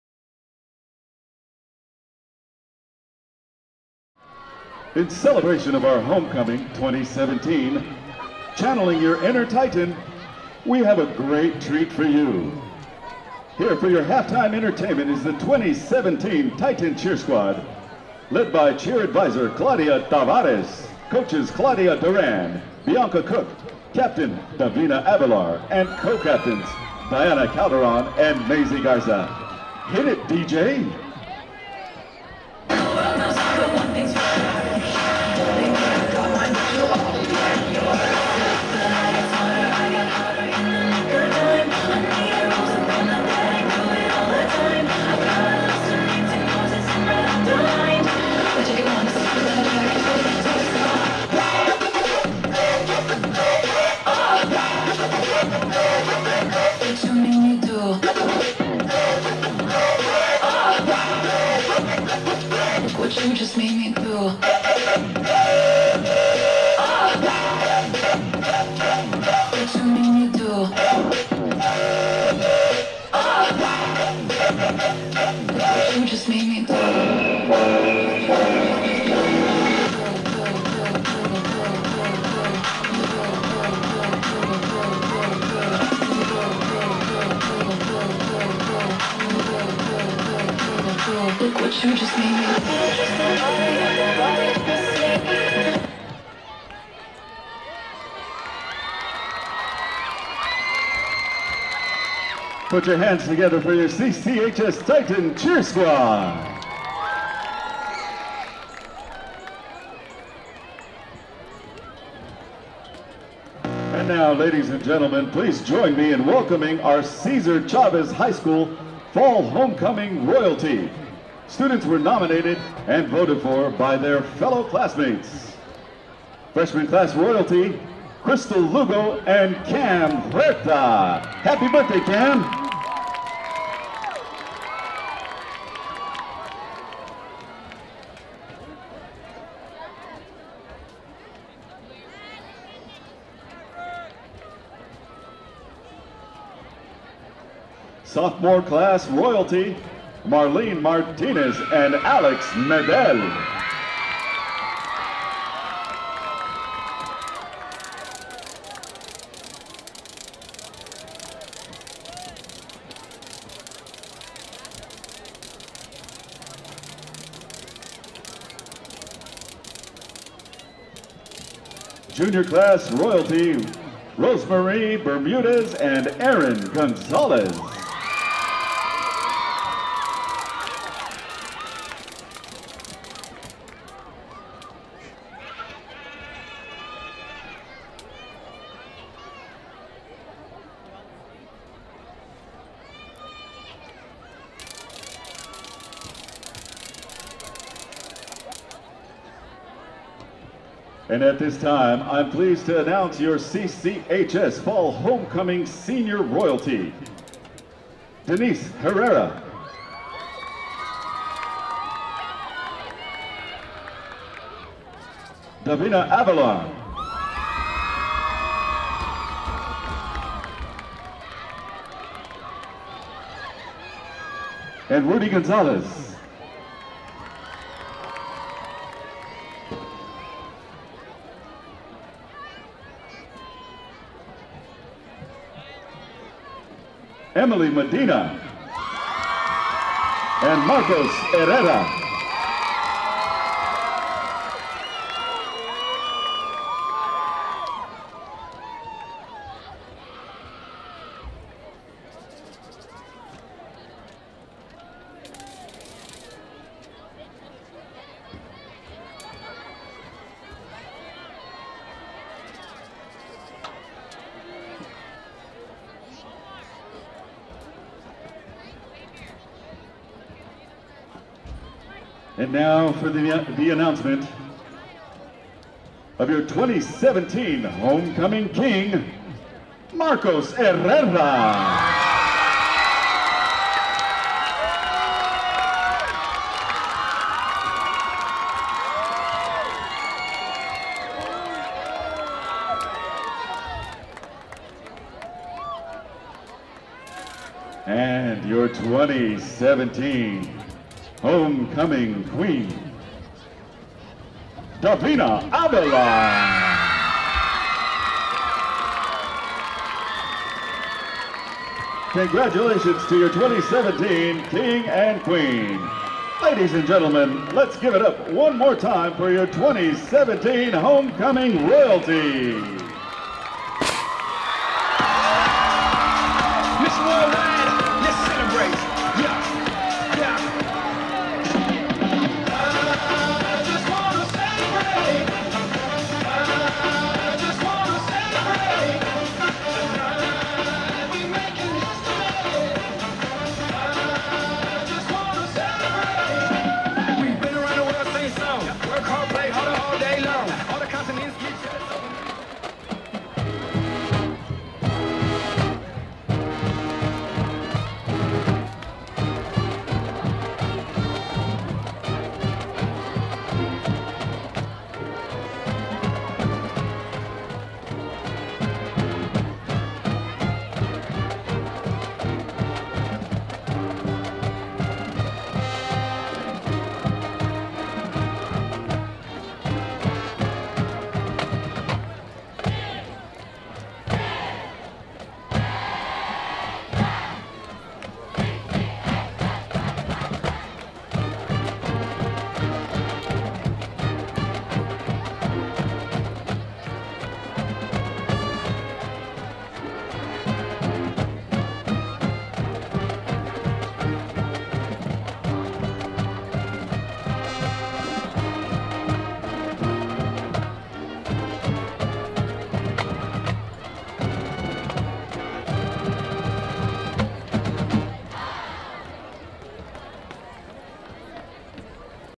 2017 Cesar E. Chavez High School Homecoming 09-29-17 part 2
Part 2 contains the cheerleader performance, the introductions and crowning of royalty, plus the fight song by the band.